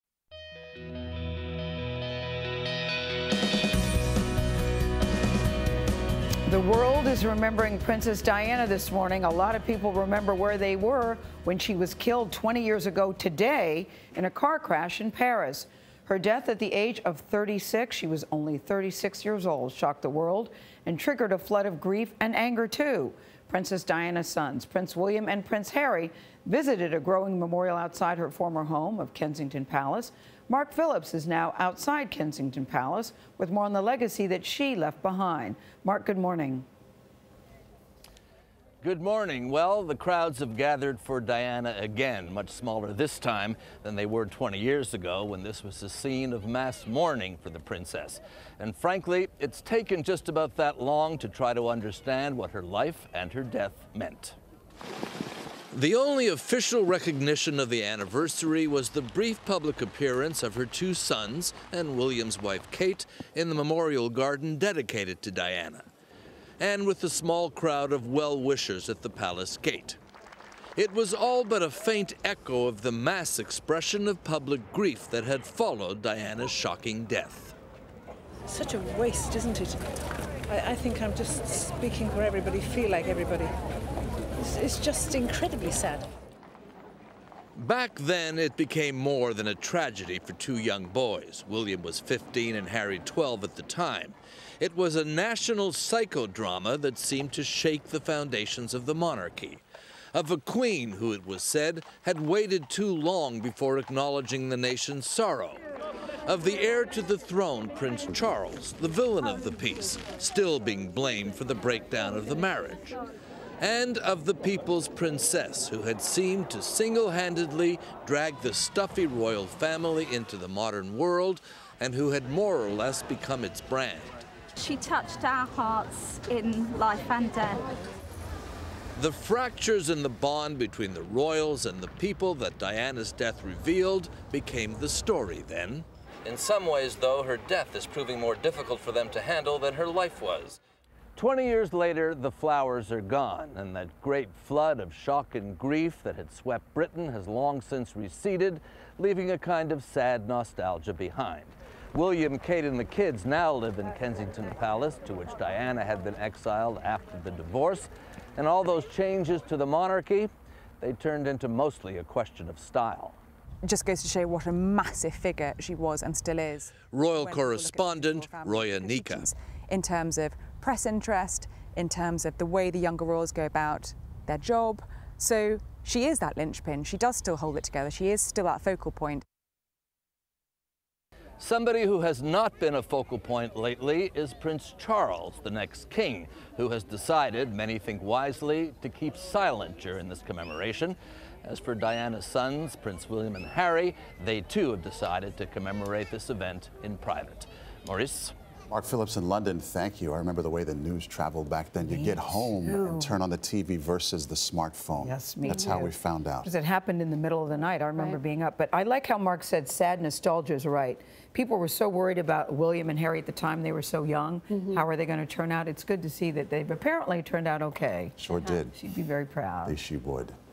Princess Diana's influence remains 20 years after her death
Thursday marks the 20th anniversary of Princess Diana's death. She was killed in a car crash at the age of 36. Her sons, William and Harry, visited a growing memorial outside of Kensington Palace. Mark Phillips reports.